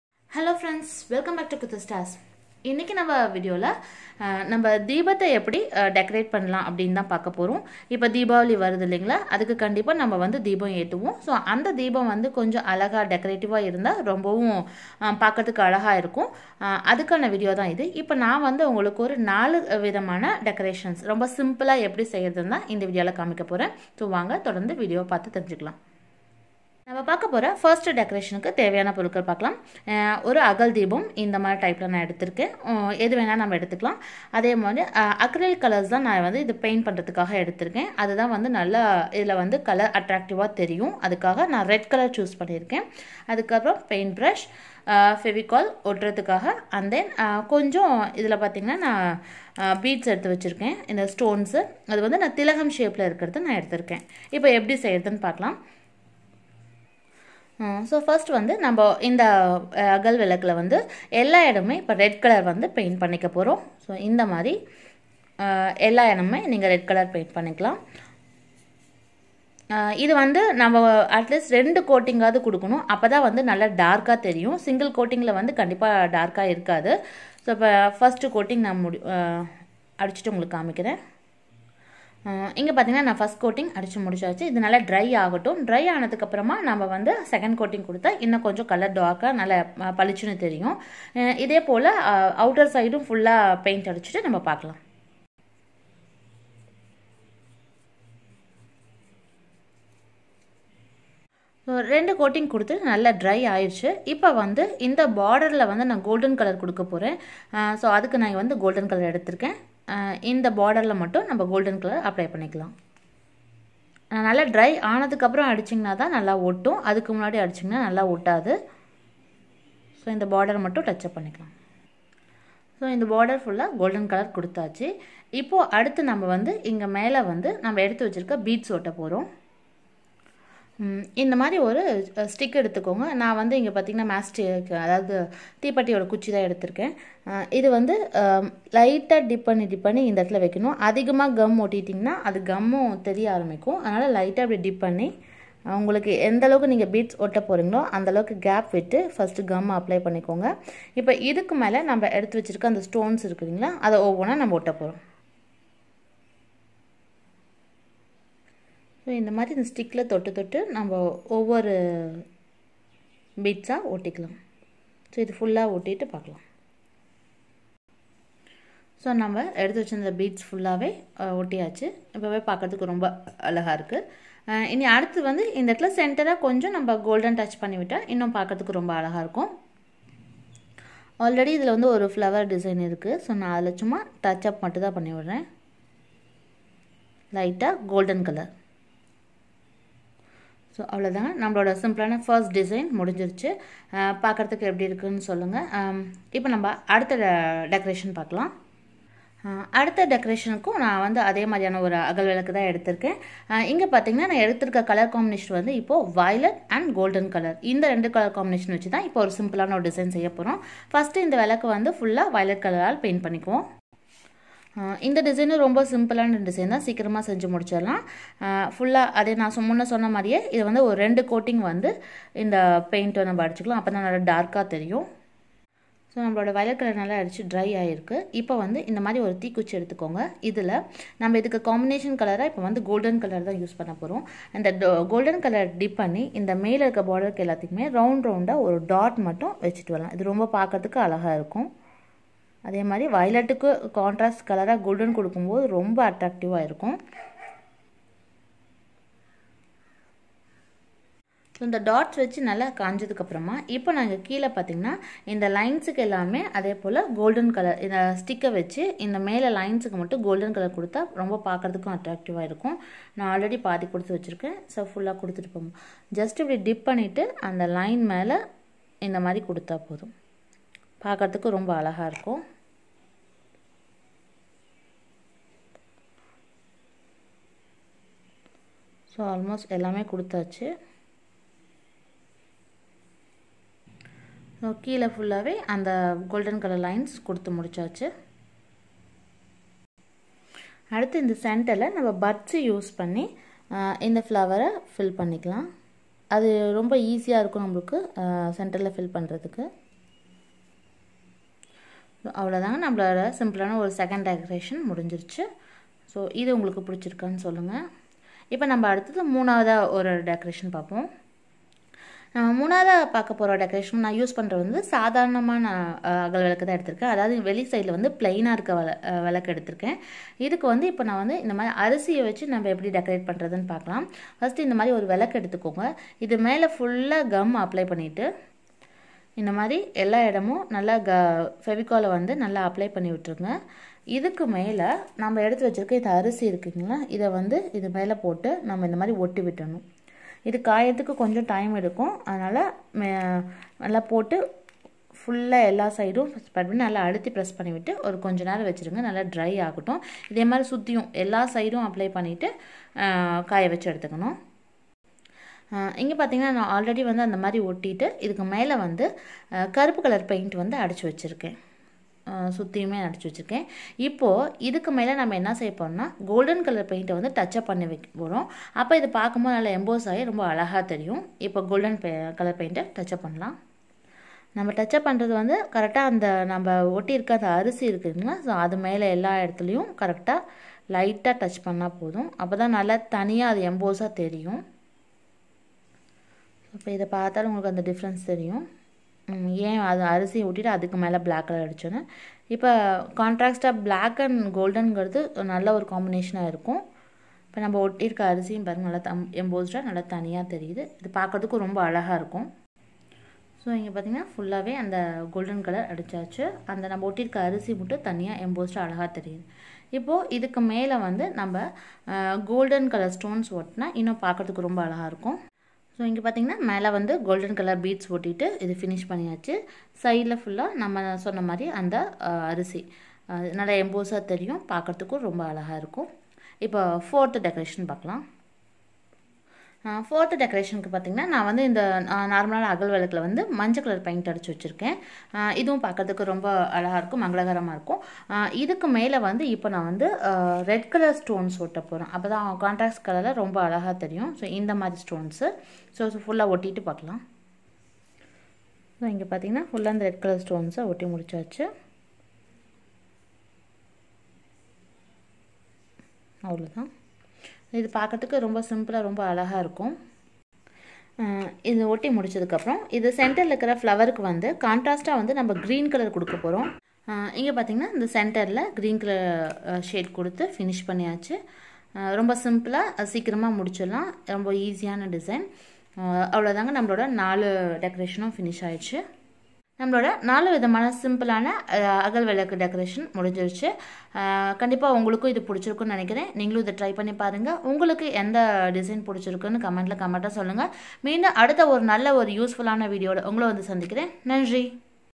4 Easy Deepam/Diya decoration at home for this Deepavali /Deepavali Decoration
In this video I shared some idea about how to decorate Deepam at home .Here I show you four simple method.so try this method and send your comments to me